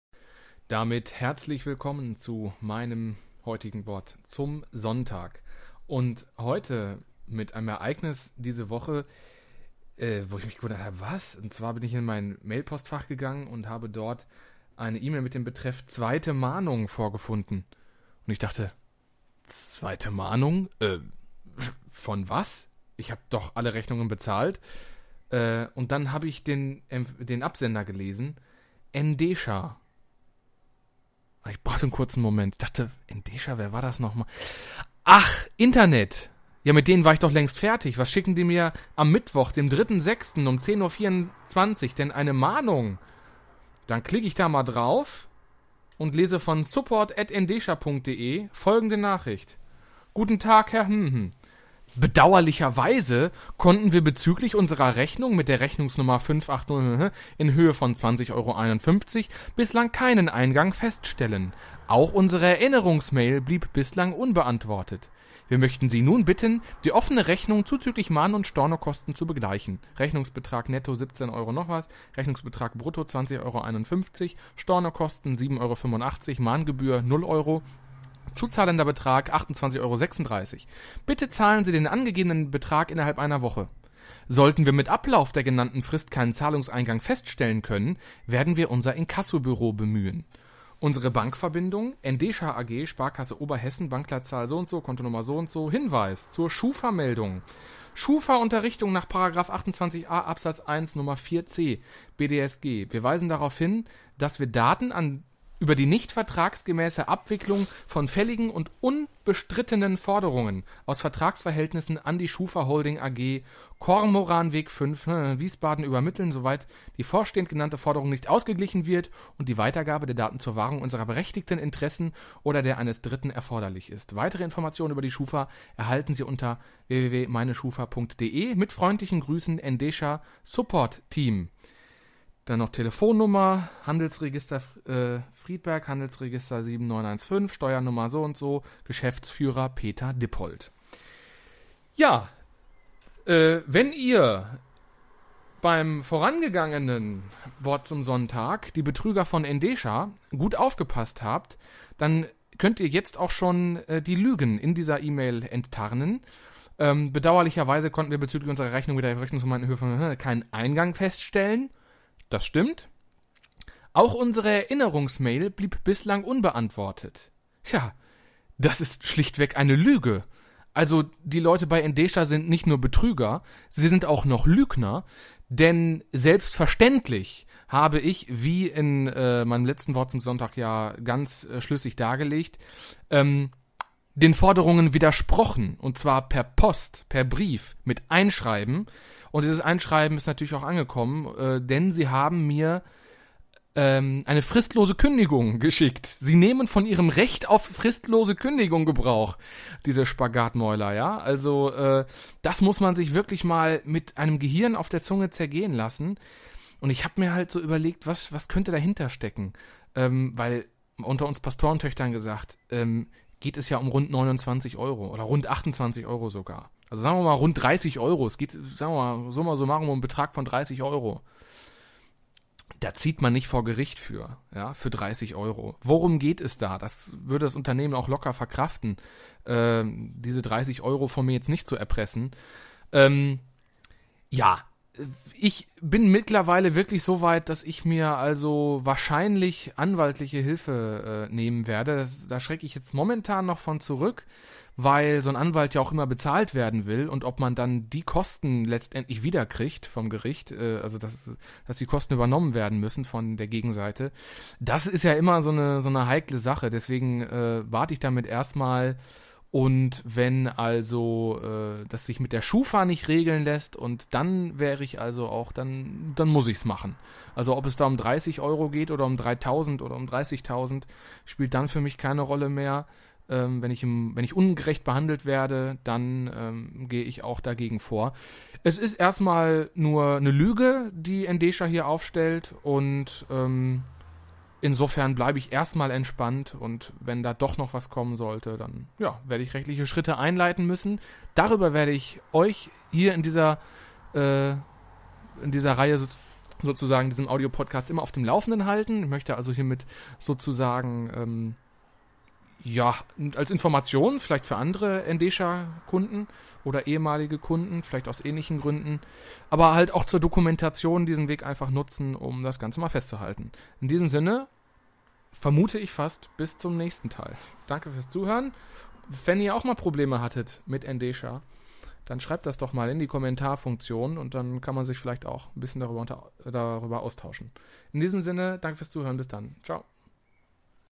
MWZS: Die Lügner von endesha | Teil 2
Es hat sich erledigt mit endesha? Nein, denn die Betrüger sind jetzt auch noch zu konkreten Lügnern mutiert!